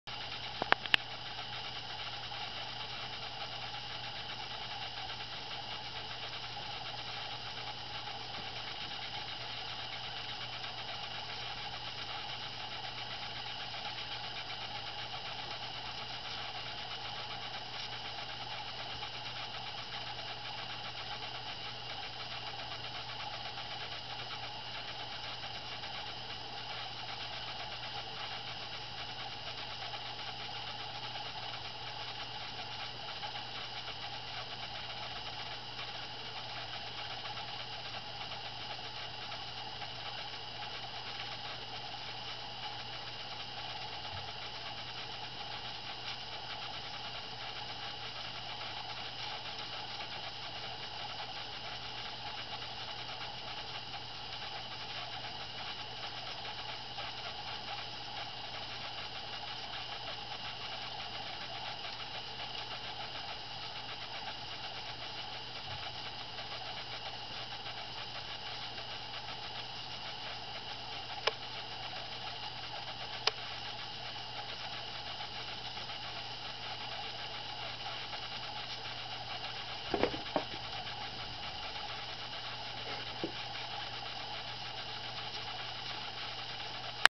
Finch chase
Sunflower seeds on woodbox--birds come and eat